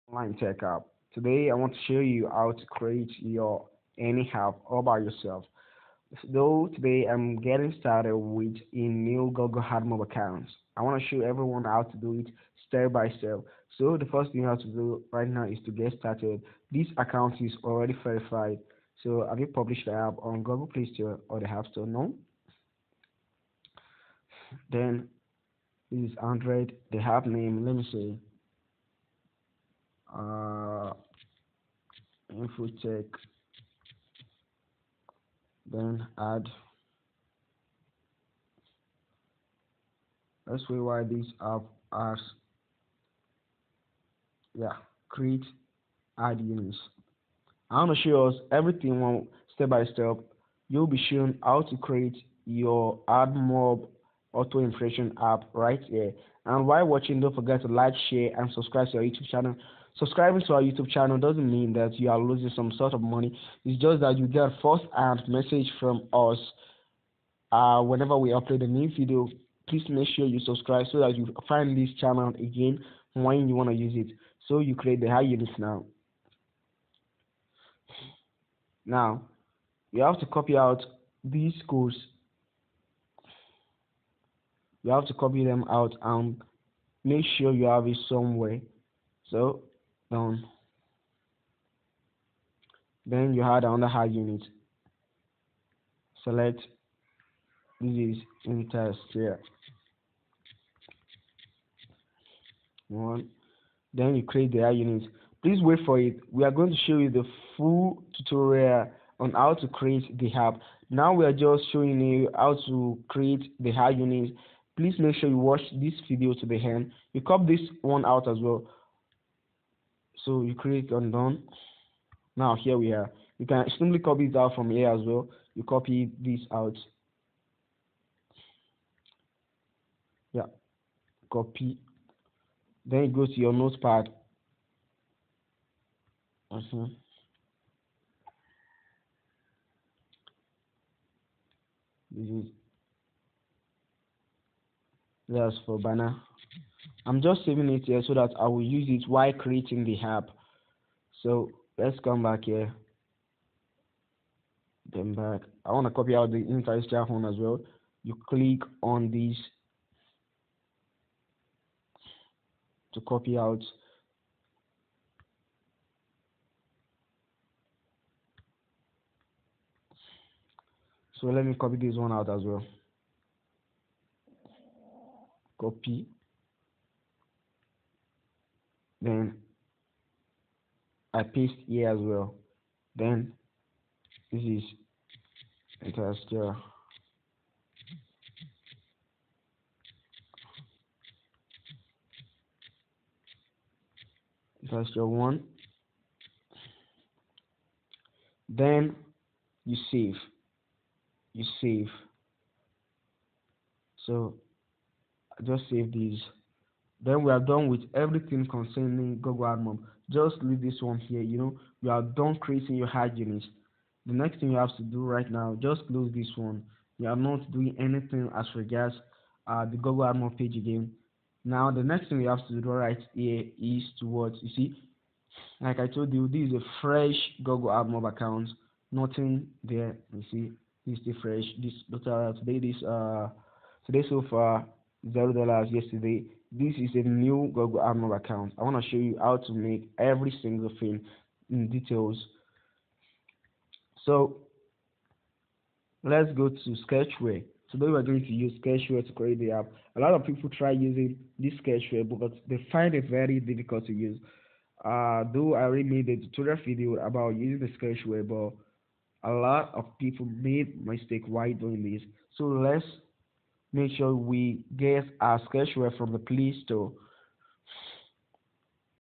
How to create Admob Auto Impression Self Clicking App part 1 (Sketchware)
How to create Admob Auto Impression Self App by your self is done in different session, below are their link.

Watch this video carefully and please don't fail to practice this till you becomes perfect.

2020 Admob Self Clicking Trick

Love ya😘